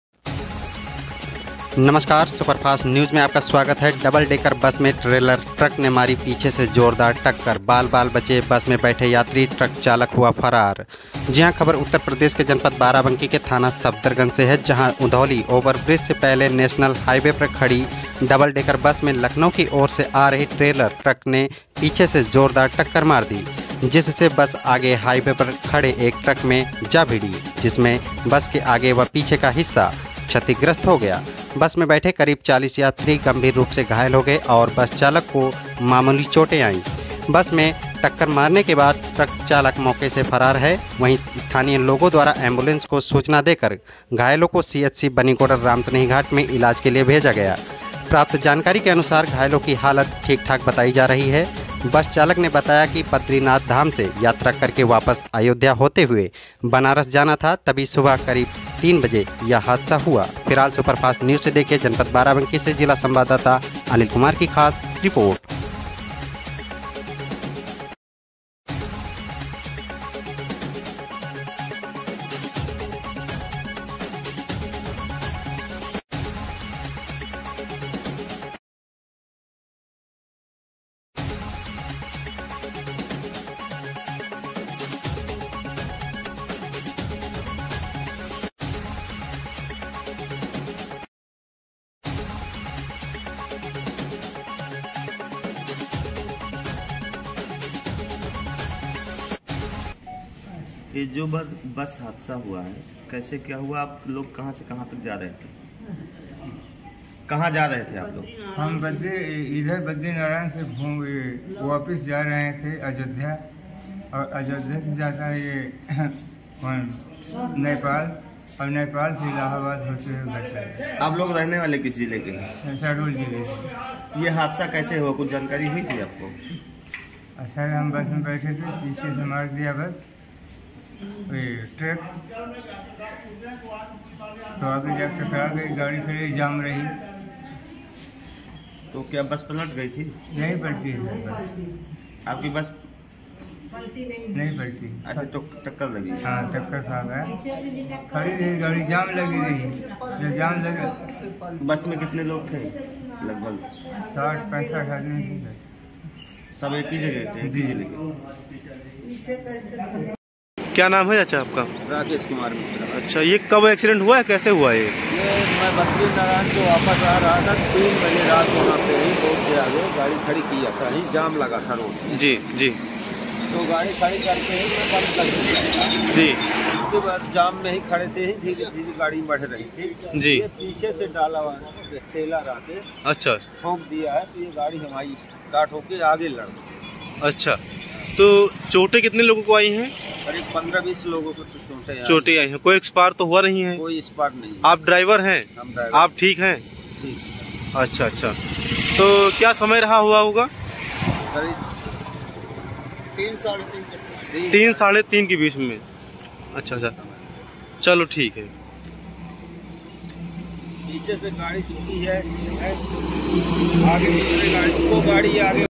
Barabankinews : डबल टेकर बस में ट्रेलर ने पीछे से मारी जोरदार टक्कर,बाल- बाल बचे बस में बैठे
Barabankinews : डबल टेकर बस में ट्रेलर ने पीछे से मारी जोरदार टक्कर,बाल- बाल बचे बस में बैठे यात्री ???

 बस में लगभग 40 यात्री हुए गम्भीर रूप से घायल,ट्रक चालक हुआ फरार ||